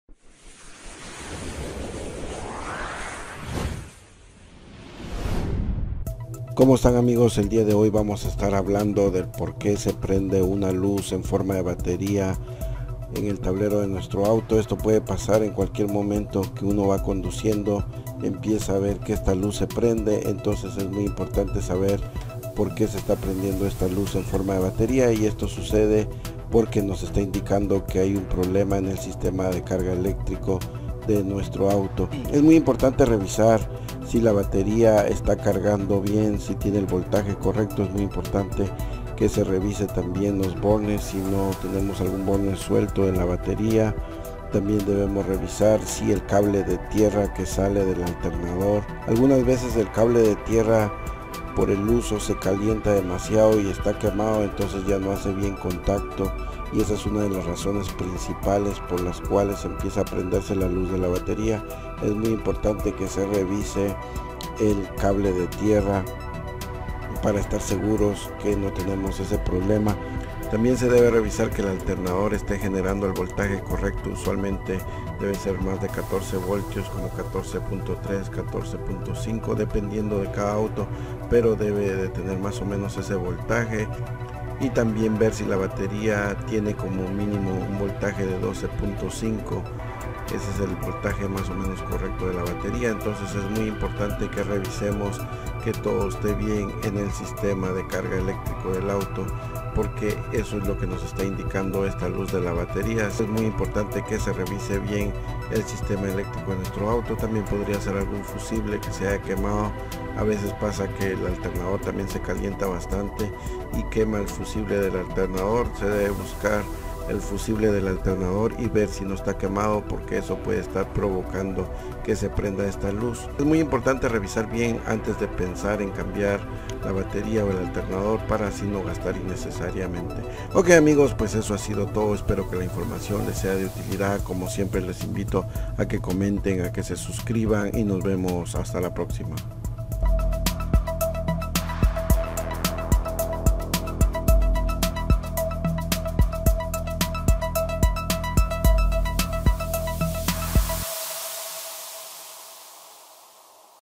Porque se Prende una luz en Forma de Batería - en el Tablero de mi Carro
En Este vídeo te explicamos las causas que provocan que se prenda repentinamente una luz en forma de batería